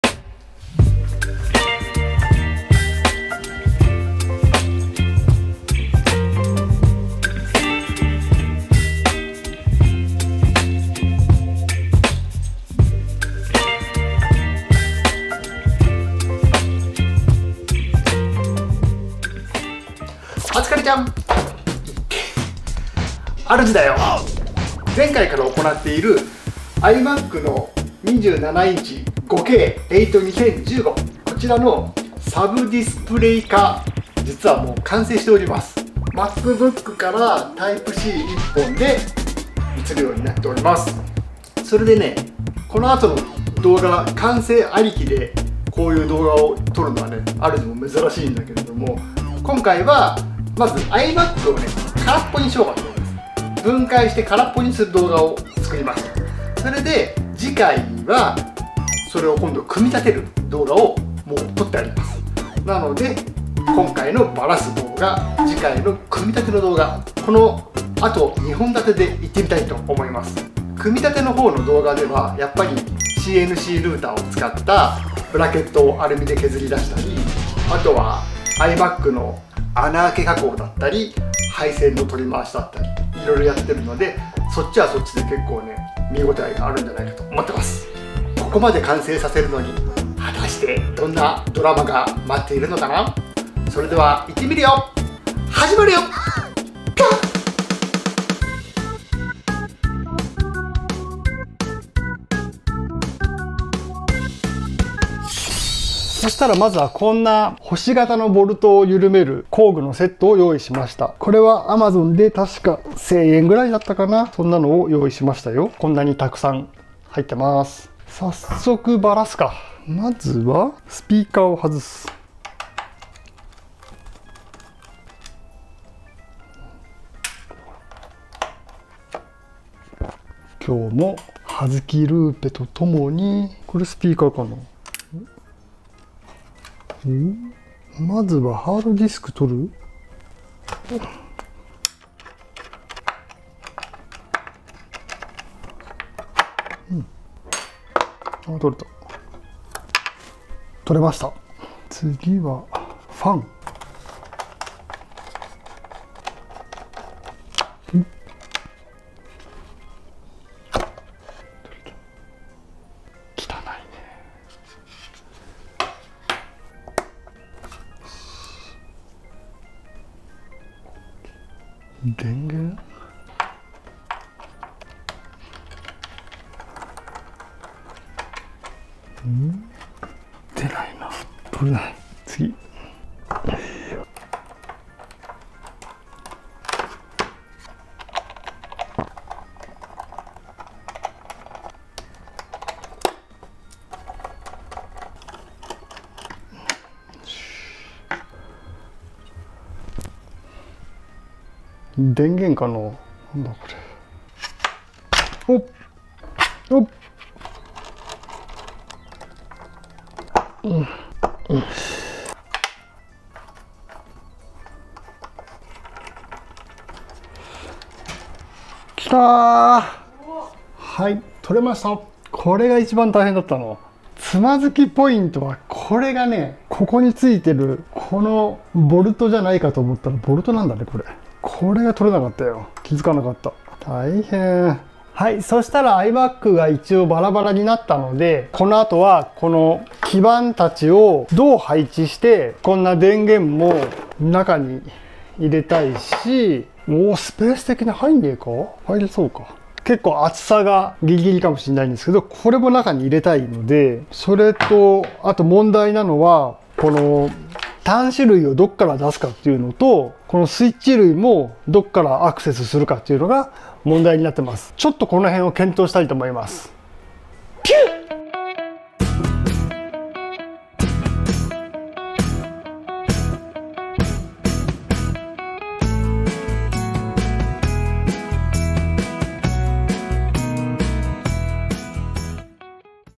iMac 27-inch Late2015 Retina 大改造、分解編！iMacをiMacに映し出す！
iMac 27-inch Late2015 を大改造、分解編！
iMac 5Kを外付けディスプレイ化するよ！

5Kモニター2台も可能だぜ！

はじまるよっ！